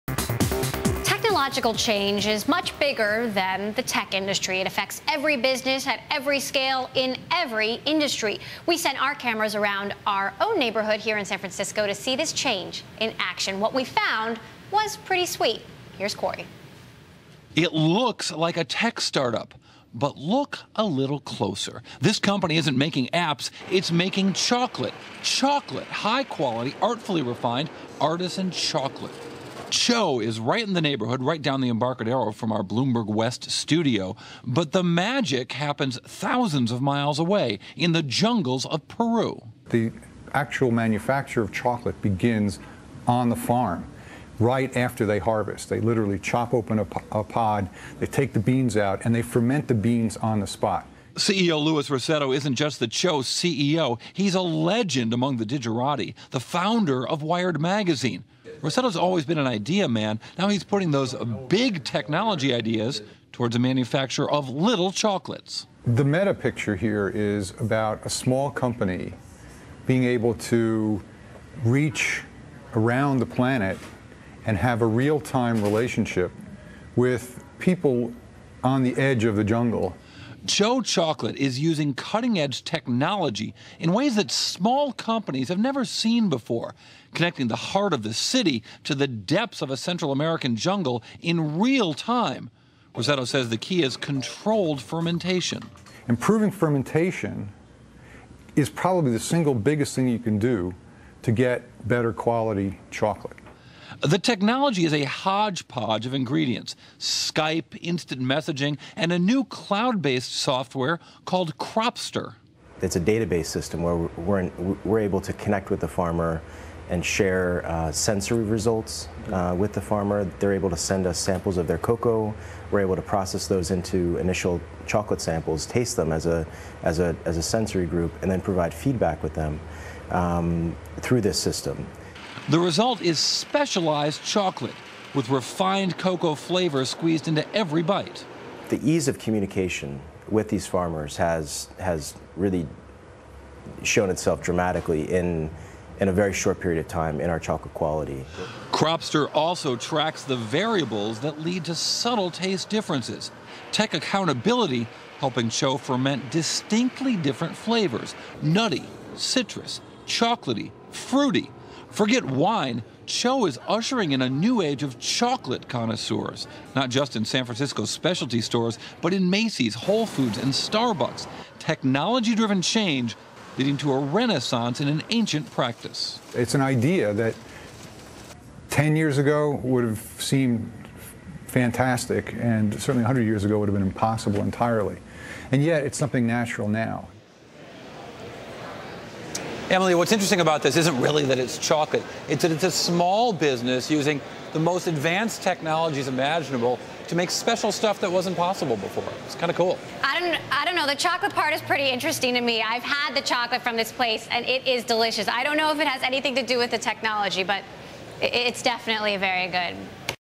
Chocolate Maker TCHO Uses Technology to Boost Quality (Video)
Oct. 19 (Bloomberg) -- Bloomberg's Cory Johnson reports on TCHO chocolate and how the San Francisco-based company involves technology in the making of its artisan chocolate. Emily Chang also speaks. (Source: Bloomberg)